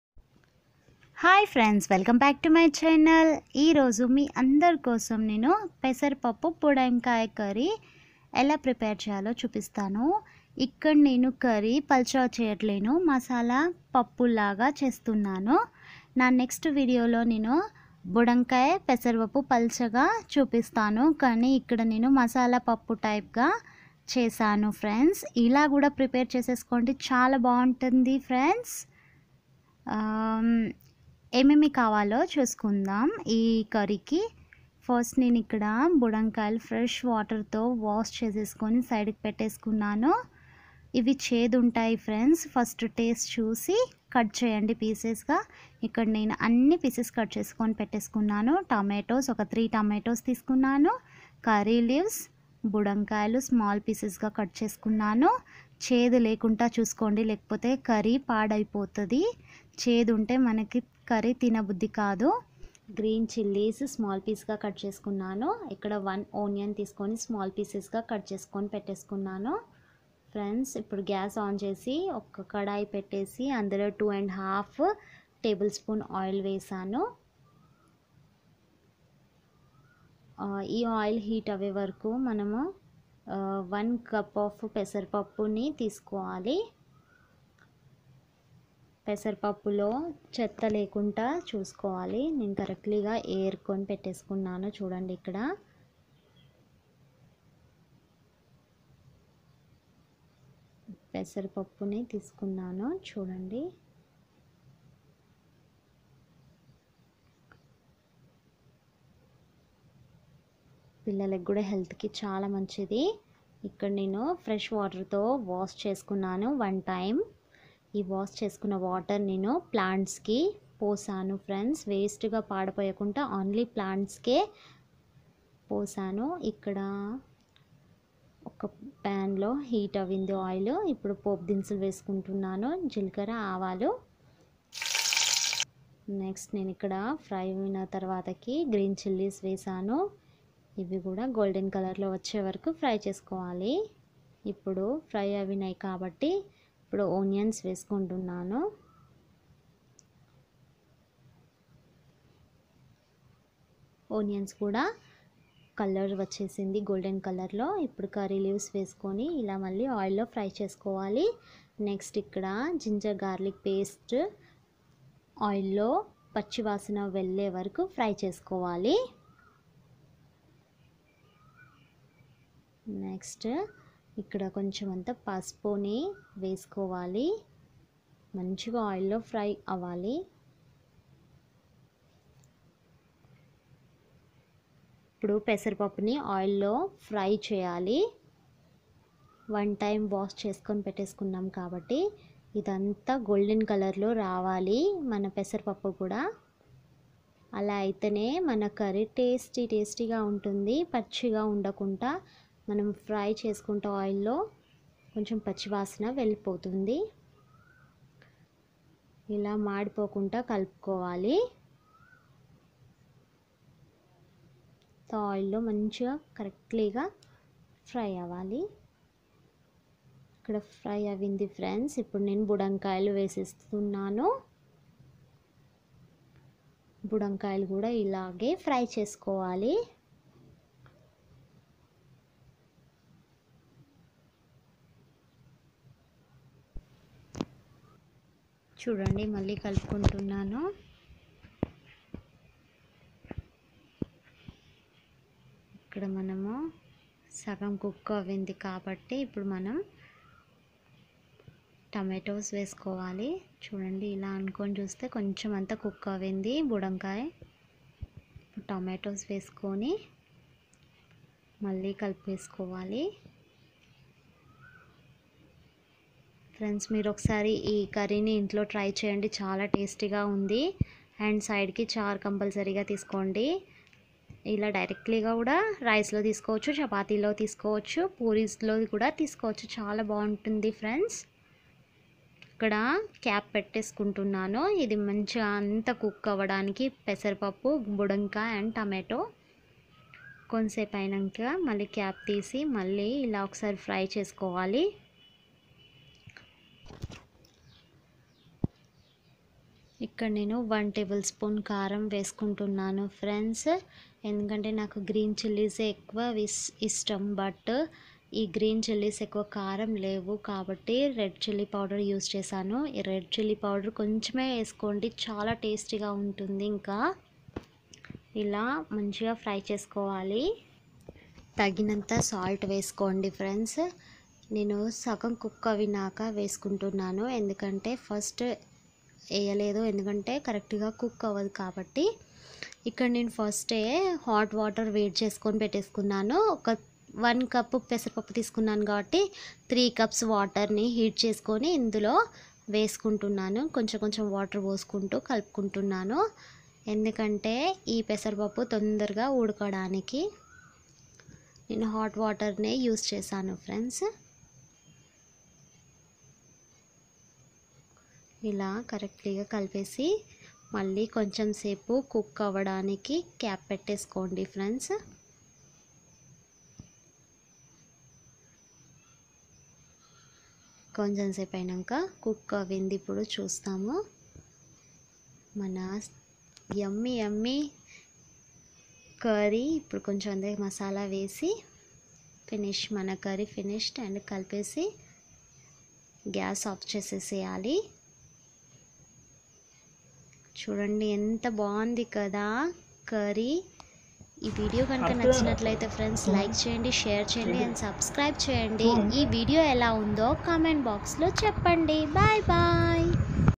How to prepare Pessarpappu Buddamkaya curry in telugu.
For all the people.
Mainly for foodies.
Bachelor's and newly married girls.